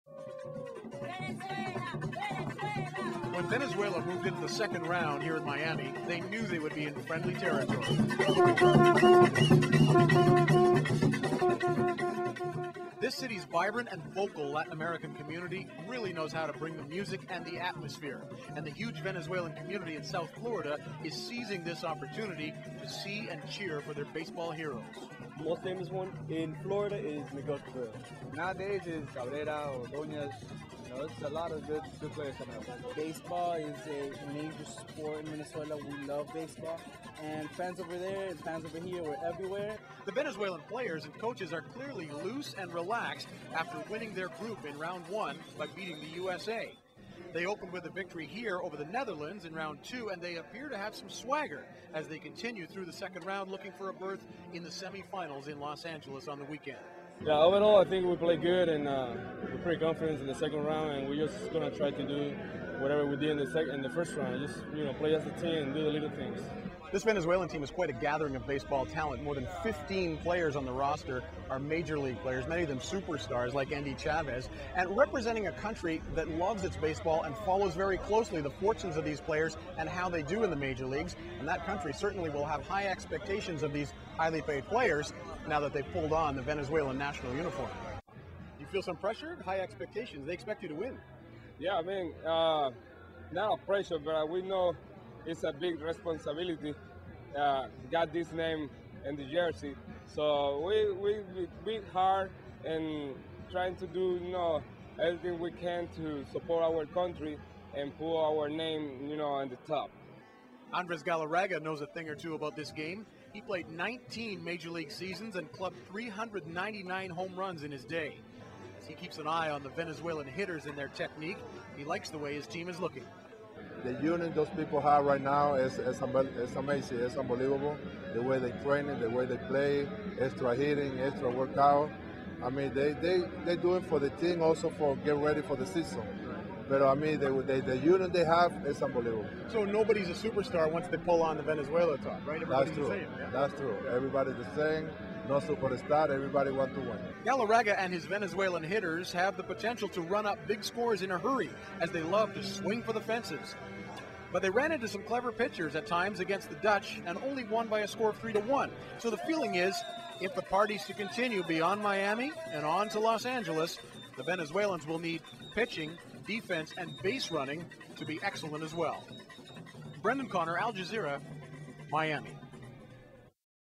VENEZUELA BASEBALL TEAM FACES HIGH EXPECTATIONS-March 2009
Al Jazeera's Brendan Connor reports from the 2009 World Baseball Classic, where Venezuela has been good, but must be great because their fans expect it.